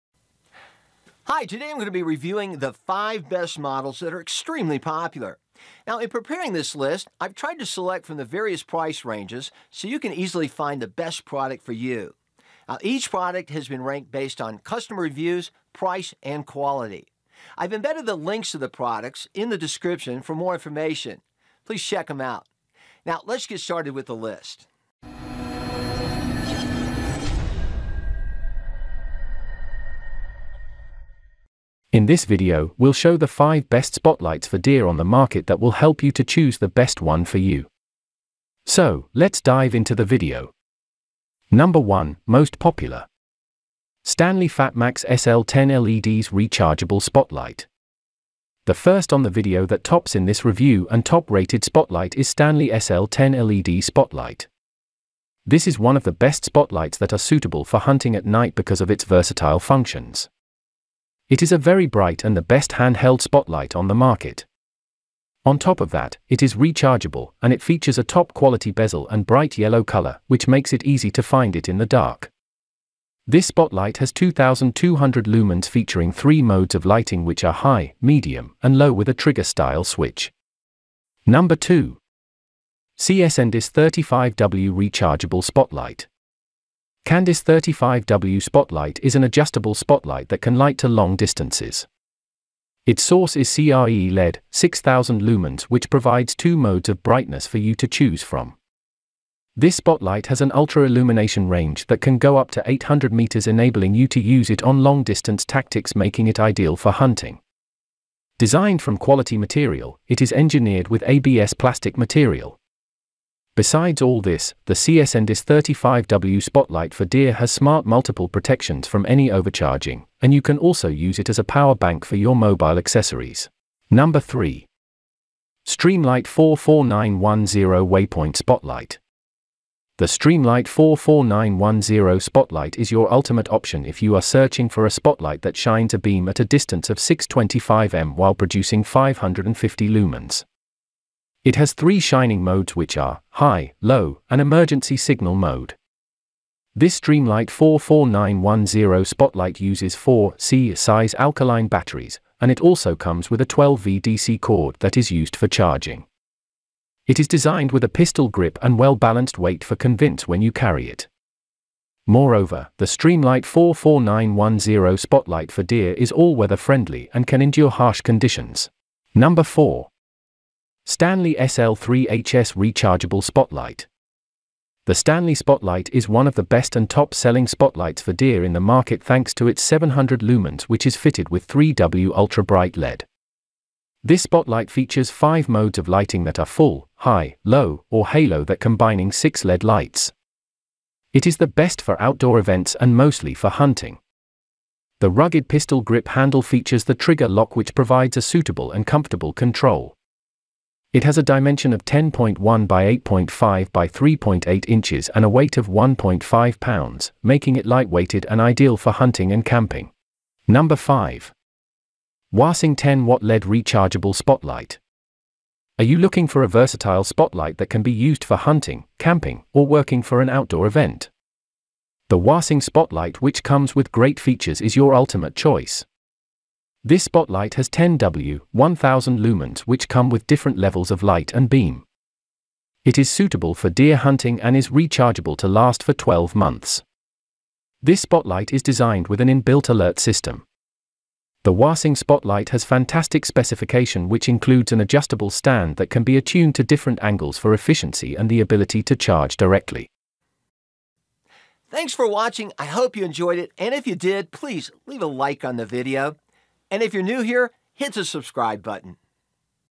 Best Spotlight for Deer in 2023 - Top 5 Review [10 Watt 1000 Lumens LED Rechargeable Spotlight]
Links to the best spotlight for deer listed in this review video:
► 1. STANLEY FATMAX SL10LEDS Rechargeable Spotlight
► 2. CSNDICE 35W Rechargeable Spotlight
► 3. Streamlight 44910 Waypoint Spotlight
► 4. STANLEY SL3HS Rechargeable Spotlight
► 5.  WASING 10 Watt LED Rechargeable Spotlight

🕝Timestamps🕝
0:00 - Introduction
0:41 - STANLEY FATMAX SL10LEDS Rechargeable Spotlight

1:21 - CSNDICE 35W Rechargeable Spotlight
2:03 - Streamlight 44910 Waypoint Spotlight
2:48 - STANLEY SL3HS Rechargeable Spotlight

3:30 - WASING 10 Watt LED Rechargeable Spotlight

★What is a spotlight for deer?
Spotlighting or lamping (also known as jacklighting, shining, illuminating, and pit-lamping) is a method of hunting nocturnal animals using off-road vehicles and high-powered lights, spotlights, lamps or flashlights, that makes special use of the eyeshine revealed by many animal species.